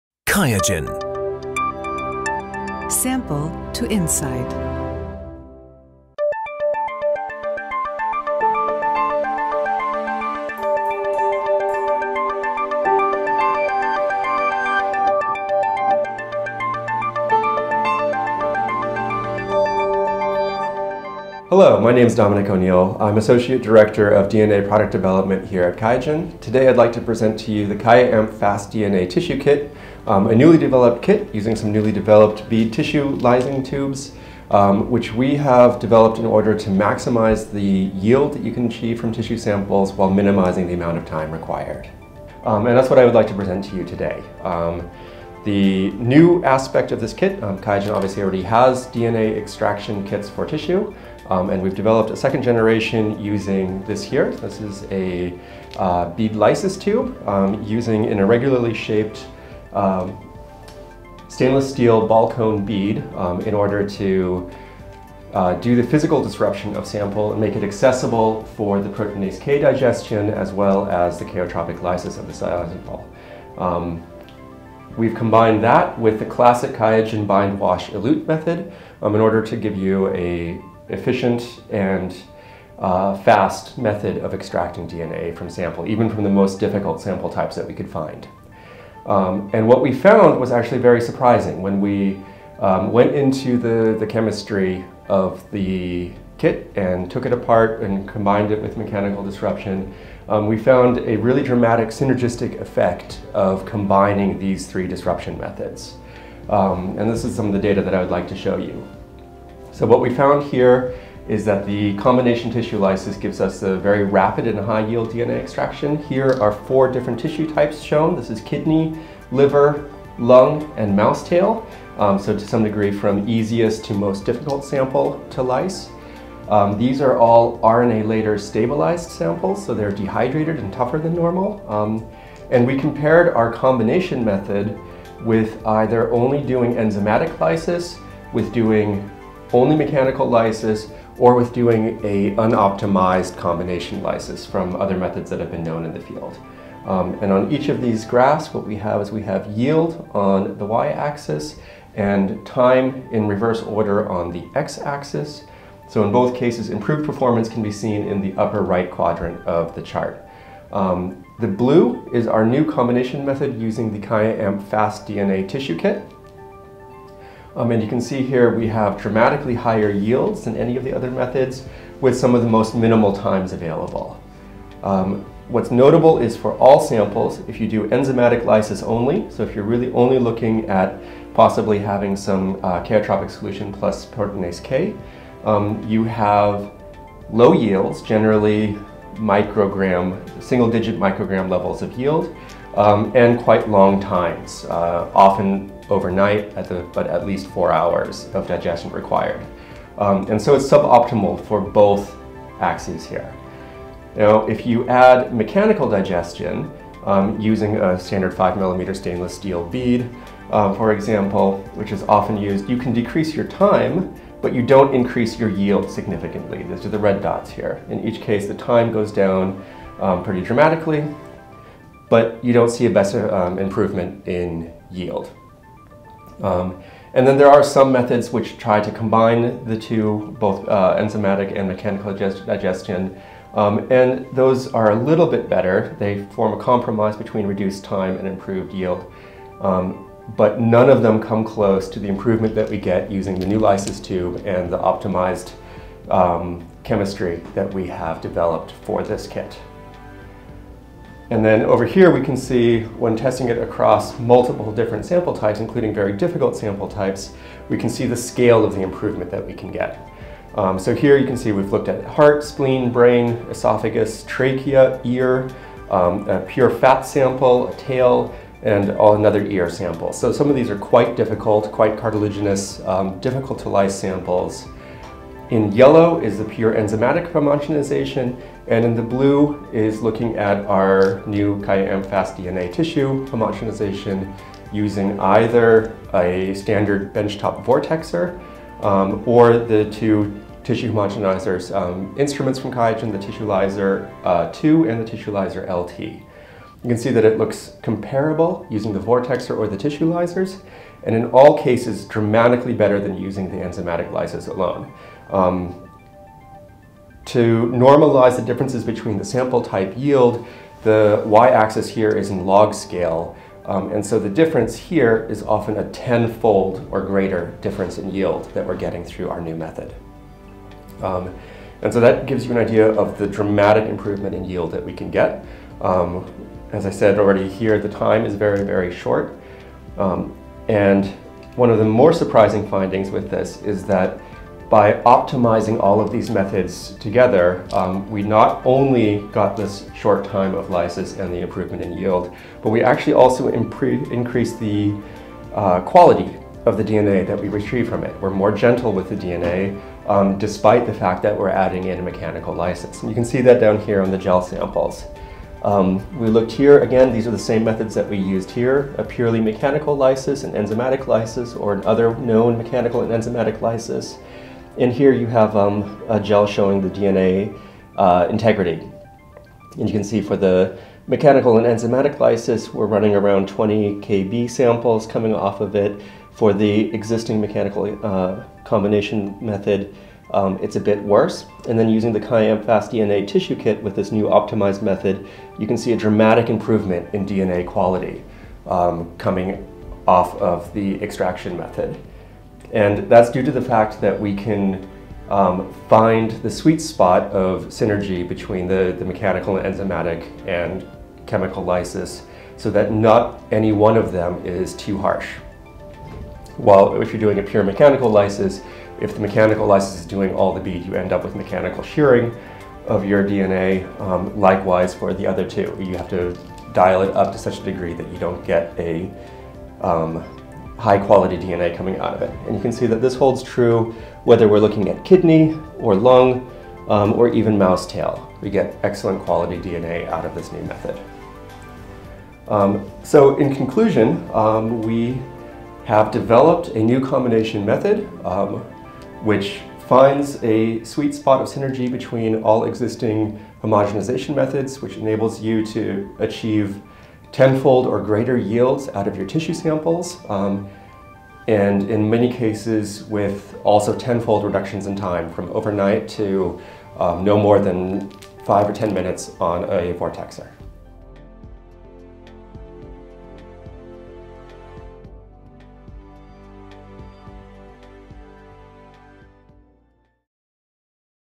Purify more DNA from challenging tissues – new faster protocol!
This new video from QIAGEN R&D explains how our latest QIAamp Fast DNA Tissue Kit delivers genomic DNA from challenging tissue samples using a rapid combination of mechanical, chemical and enzymatic lysis. It is proven to yield 5 to 10 times more genomic DNA than other kits.